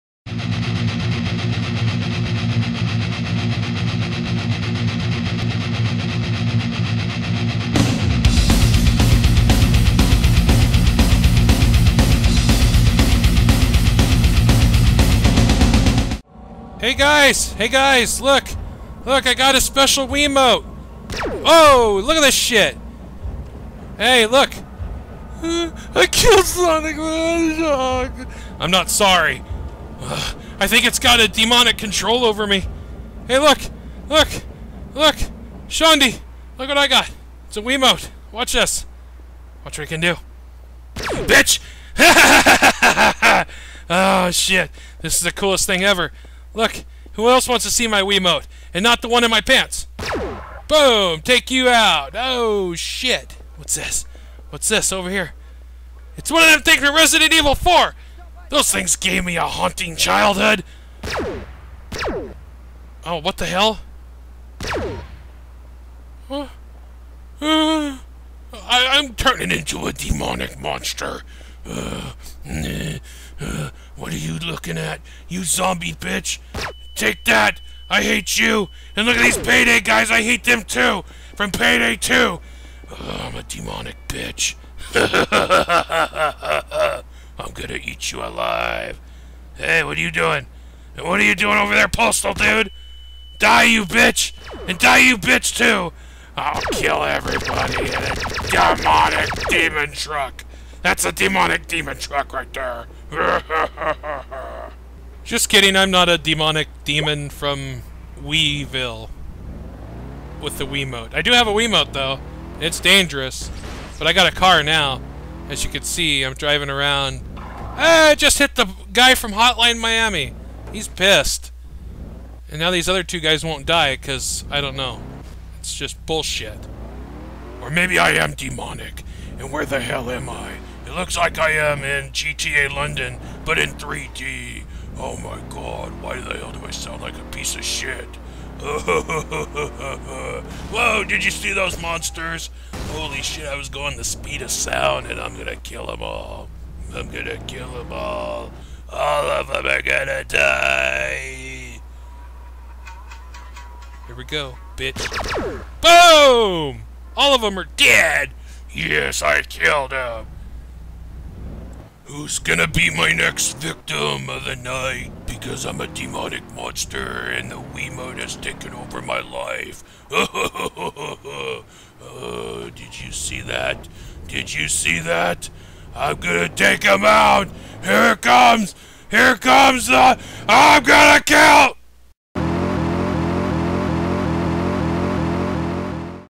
Heavy Metal Gamer Presents: Wiimote Killed My PC Dead
You are probably looking at the title, and thinking what the hell is this? Well sometimes I don't know, but this is episode 2 of my little mini series "Heavy Metal Gamer Presents". Alcohol may have played a factor in this ridiculous episode. I hope you enjoy this video.